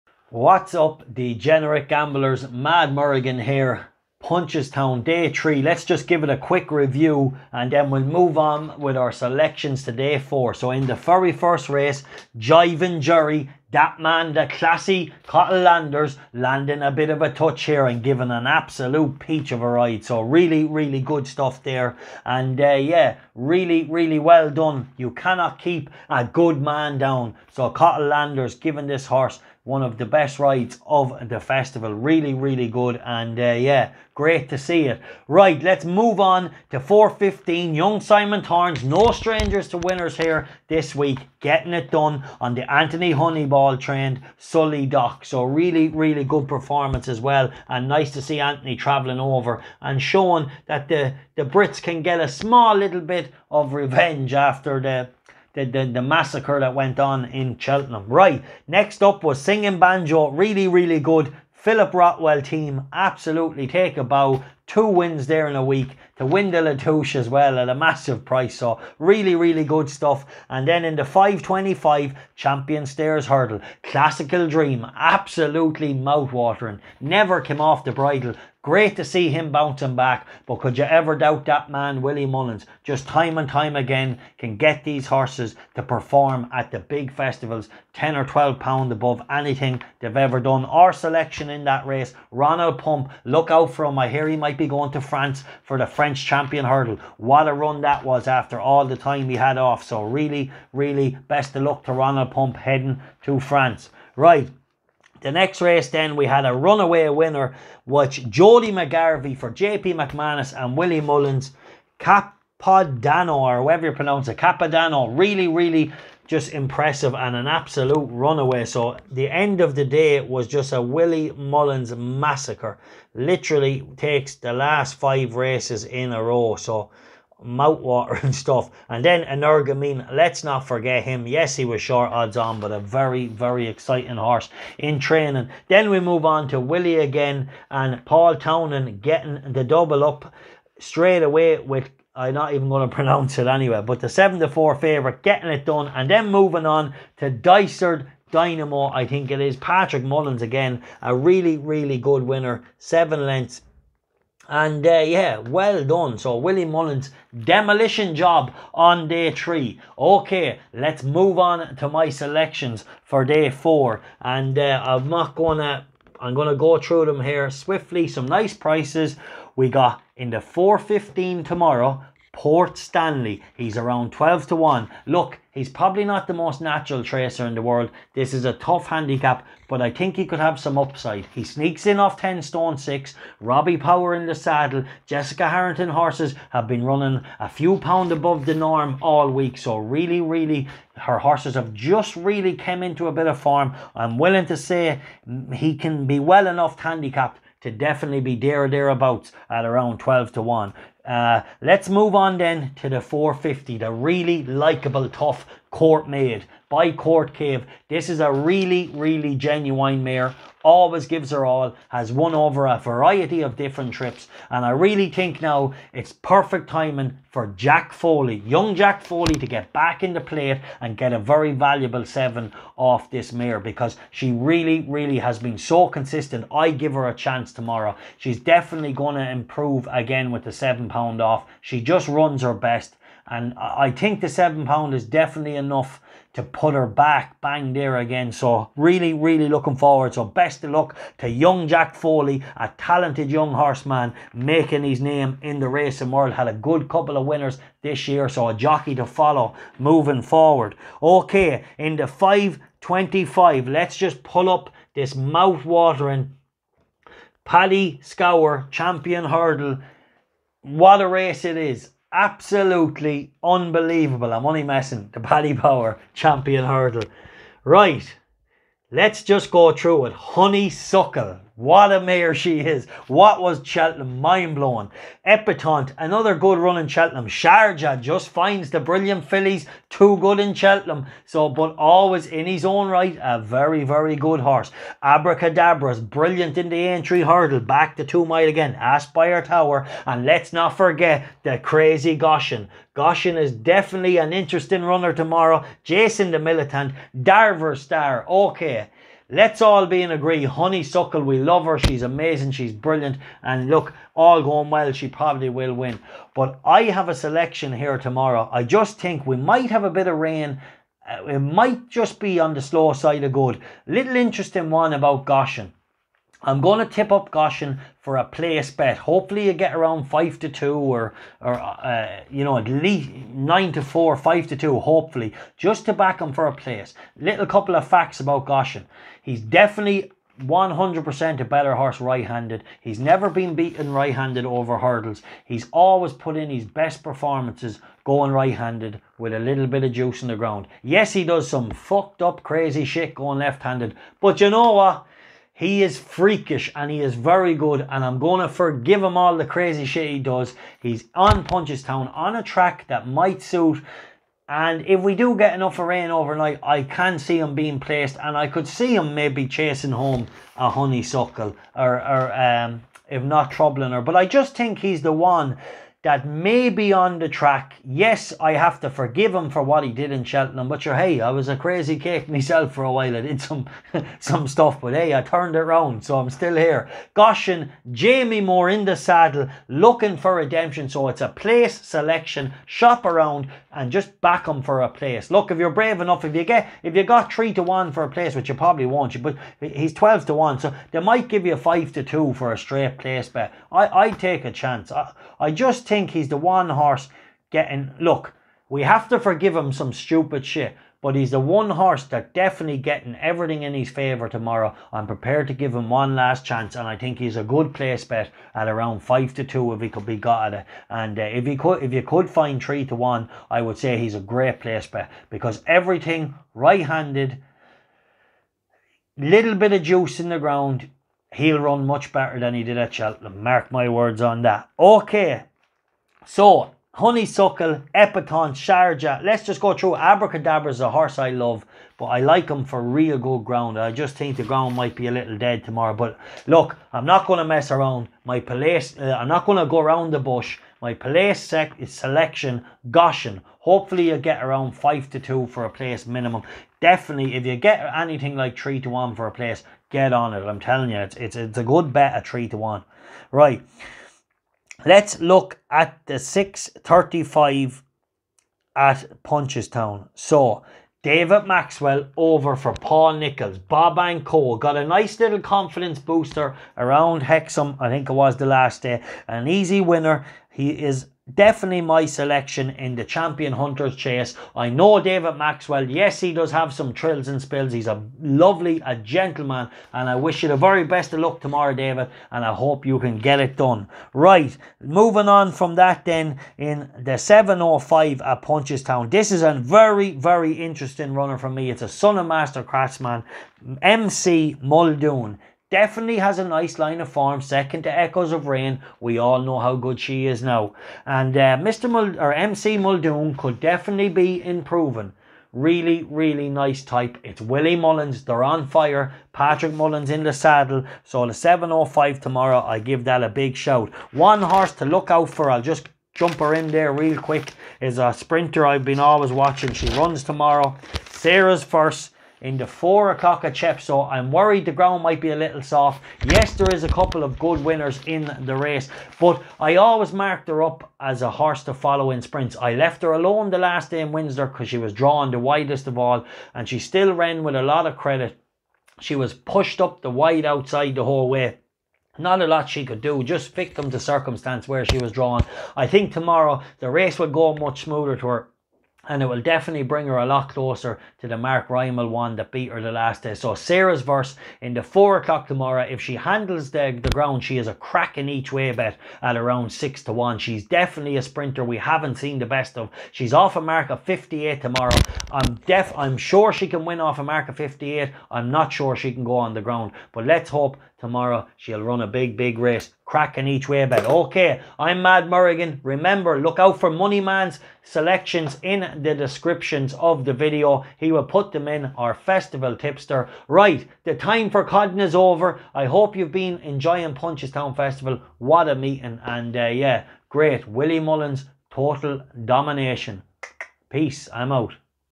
Horse Racing Tips | Punchestown Day four | Mad Merrigan | Coldbeer - Sports🏇
🏆𝗨𝗽𝗱𝗮𝘁𝗲𝗱 𝗪𝗶𝗻𝗻𝗲𝗿 - 𝗕𝗼𝗯 𝗮𝗻𝗱 𝗖𝗼 𝟯/𝟭 𝗼𝗳 𝗛𝘂𝗻𝘁𝗲𝗿𝘀 𝗖𝗵𝗮𝘀𝗲🤑🏆🏇 𝗗𝗲𝘁𝗮𝗶𝗹𝗲𝗱 𝗣𝘂𝗻𝗰𝗵𝗲𝘀𝘁𝗼𝘄𝗻 𝘀𝗲𝗹𝗲𝗰𝘁𝗶𝗼𝗻𝘀  𝗳𝗼𝗿 𝗙𝗶𝗿𝗱𝗮𝘆 𝟮𝟵𝘁𝗵  𝗔𝗽𝗿𝗶𝗹 𝟮𝟬𝟮𝟭 | 𝗠𝗮𝗱 𝗠𝗲𝗿𝗿𝗶𝗴𝗮𝗻 | 𝗖𝗼𝗹𝗱𝗯𝗲𝗲𝗿 𝘀𝗽𝗼𝗿𝘁𝘀 🏇

𝗠𝗼𝗻𝗲𝘆 𝗠𝗮𝗻 𝗦𝗲𝗹𝗲𝗰𝘁𝗶𝗼𝗻𝘀 𝗯𝗲𝗹𝗼𝘄 - 𝗠𝗮𝗱 𝗠𝗲𝗿𝗿𝗶𝗴𝗮𝗻 𝘀𝗲𝗹𝗲𝗰𝘁𝗶𝗼𝗻𝘀 𝗶𝗻 𝘃𝗶𝗱𝗲𝗼 🏇

🏆𝗨𝗽𝗱𝗮𝘁𝗲𝗱 - 𝗪𝗶𝗻𝗻𝗲𝗿 - 𝗕𝗼𝗯 𝗮𝗻𝗱 𝗖𝗼 𝟯/𝟭 & 𝗽𝗼𝗿𝘁 𝗦𝘁𝗮𝗻𝗹𝗲𝘆 𝗣𝗹𝗮𝗰𝗲𝗱 𝟭𝟮/𝟭 🏇

𝟰.𝟭𝟱 - 𝗘𝗠𝗦 𝗖𝗼𝗽𝗶𝗲𝗿𝘀 𝗡𝗼𝘃𝗶𝗰𝗲 𝗛𝗮𝗻𝗱𝗶𝗰𝗮𝗽 𝗖𝗵𝗮𝘀𝗲

𝗙𝗮𝗻 𝗗𝗲𝘀 𝗕𝗹𝘂𝗲𝘀 𝟭𝟮/𝟭 𝗲𝗮𝗰𝗵 𝘄𝗮𝘆 - He is a horse i have been waiting to pop up for some time. He is back in novice company first the first time in a while. He has been campaigned with Punchestown and Galway in mind with willie slowly bringing him to the boil while learning his trade.

This horse has talent and I cant quite be sure if tomorrow or Galway is his main target, so with that in mind, he is an each-way play at 12/1 with bet 365 paying 4 places. He is shorter elsewhere. so if you like him take his price now.

𝟱.𝟮𝟱 - 𝗣𝗮𝗱𝗱𝘆 𝗣𝗼𝘄𝗲𝗿 𝗖𝗵𝗮𝗺𝗽𝗶𝗼𝗻 𝗛𝘂𝗿𝗱𝗹𝗲
It would be madness to take on Honeysuckle and Rachael Blackmore, but she is not playable at these insane odds. She could easily back up her win at Cheltenham. I was on Goshen at Cheltenham and at these odds, he is a must for me for a place.

 You have to believe he hated Cheltenham because that run was disastrous. Moore loves this horse and there not coming over just to make up the numbers. There is the small matter of a huge bonus if he beats her. I'm not saying that but Moore will have him fired up and we will see a different 𝗚𝗼𝘀𝗵𝗲𝗻 𝘁𝗼𝗱𝗮𝘆 𝗮 𝘀𝗺𝗮𝗹𝗹 𝗲𝗮𝗰𝗵-𝘄𝗮𝘆 𝗽𝗹𝗮𝘆 𝗮𝘁 𝟭𝟮/𝟭 𝘄𝗶𝘁𝗵 𝗕𝗲𝘁 𝟯𝟲𝟱

𝗔𝘂𝘁𝗵𝗼𝗿 – 𝗠𝗼𝗻𝗲𝘆 𝗠𝗮𝗻 🤑– 𝗖𝗼𝗹𝗱𝗯𝗲𝗲𝗿 𝗦𝗽𝗼𝗿𝘁𝘀 🏇